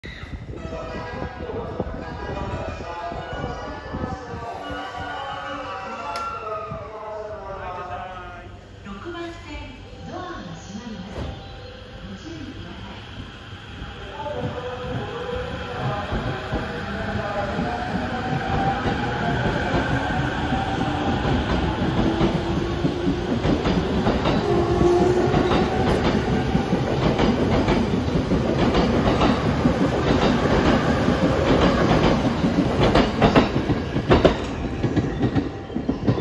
【着雪多数有り！大雪の中を駆ける！】E231系500番台ﾐﾂA501編成（トップナンバー）が中央総武線各駅停車千葉行きとして中野駅5番線を発車するシーン（2106B）2024.2.5.22:04
良きねえ